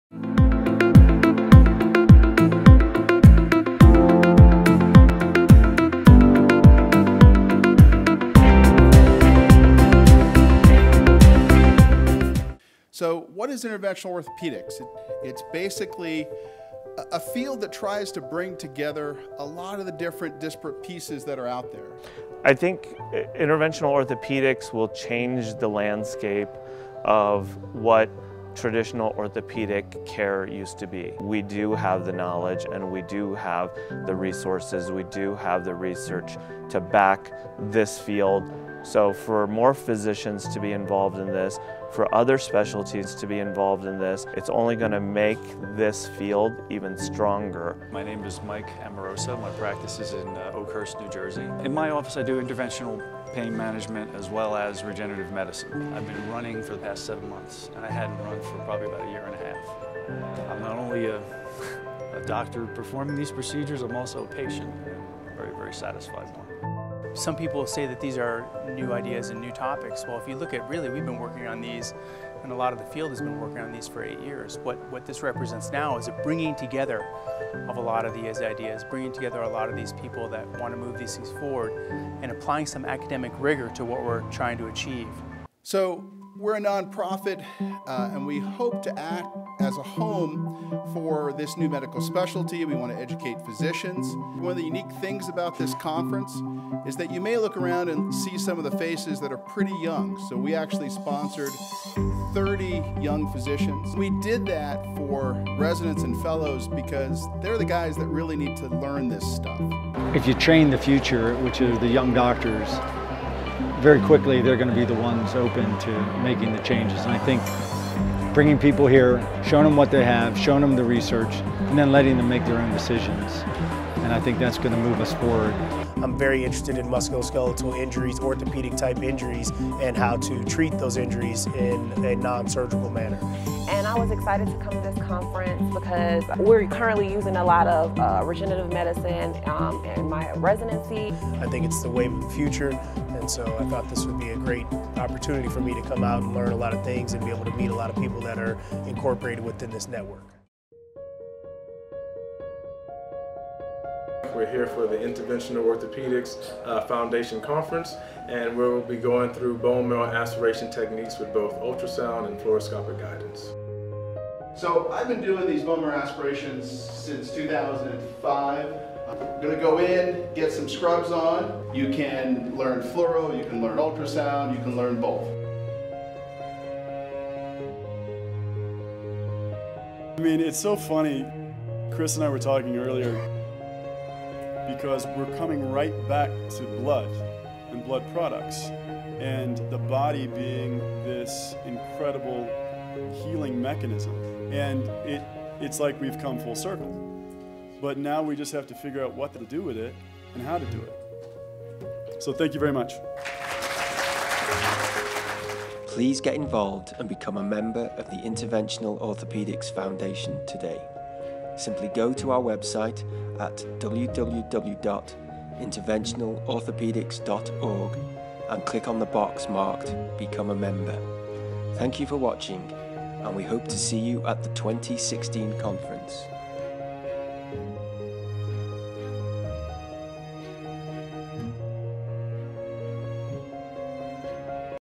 The Annual Interventional Orthopedics Conference
Overview of the IOF Conference. If you are a doctor interested in regenerative medicine, register for the February 2017 conference in Broomfield, Colorado